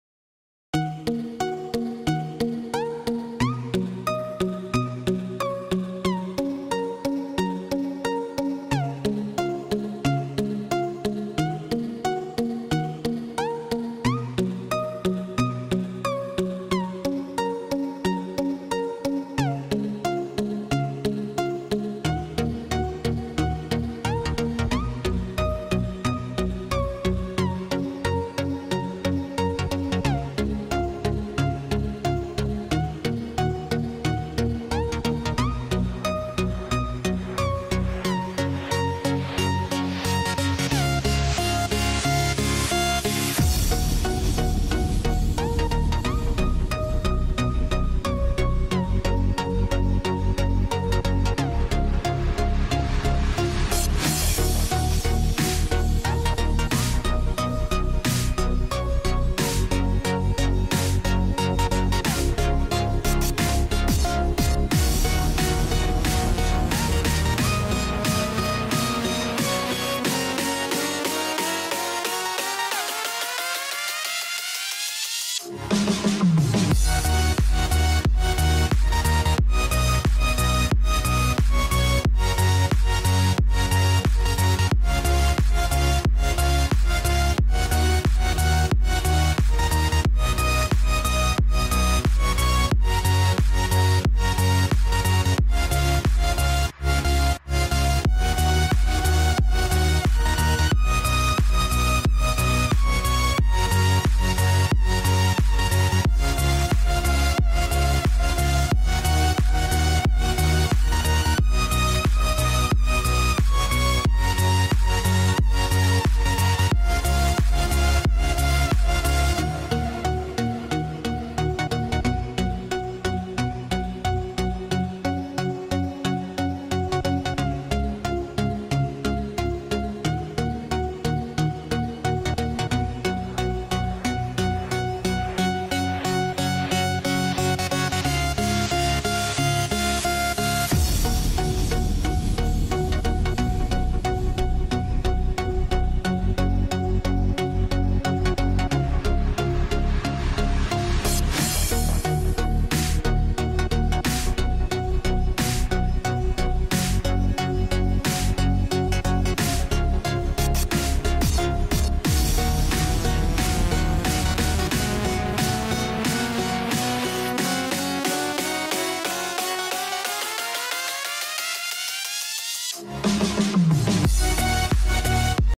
How Cup Noodles Are Made - Bul Dak Bok Um Ramen
Find out how cup noodles are made
It is made amazingly through an automated process in the factory.